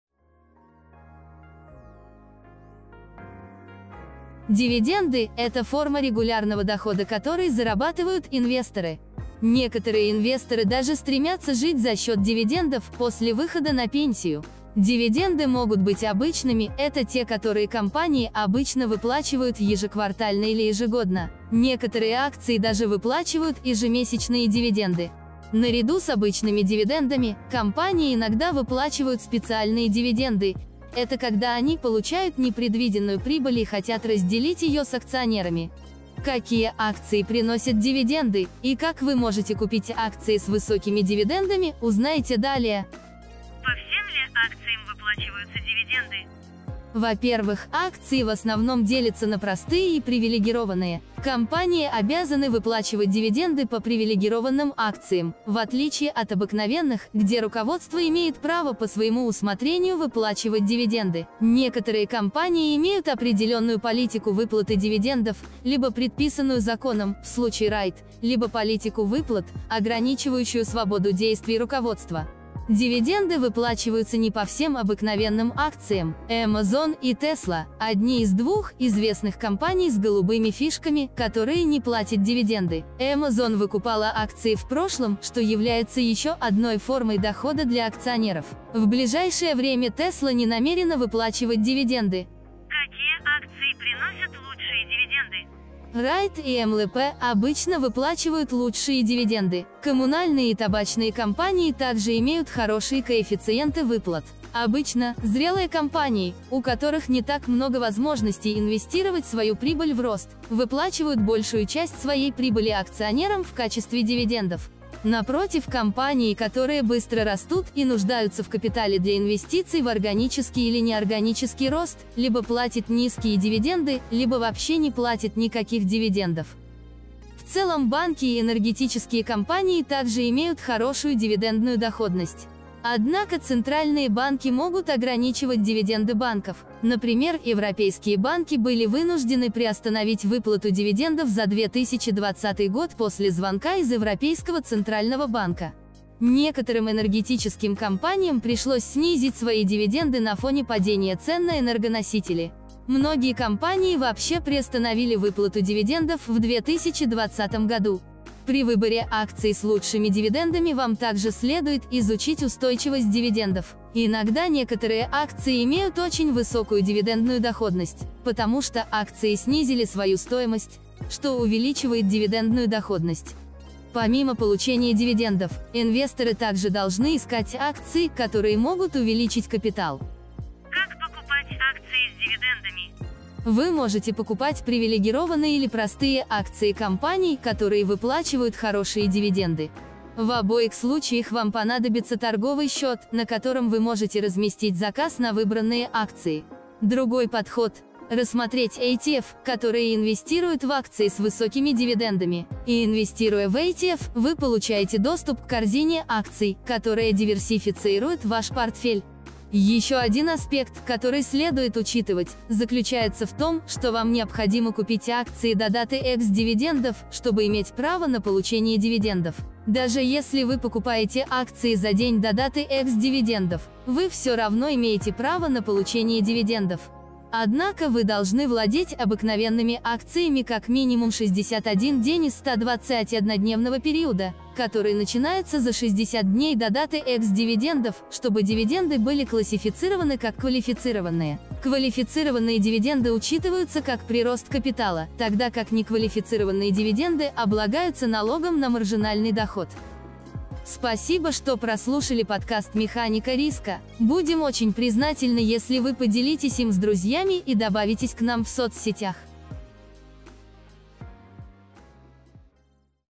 МЕХАНИКА РИСКА #11 ПОДКАСТ | Какие акции выплачивают дивиденды и как их купить?
Наш подкаст доступен на площадках Google и Apple .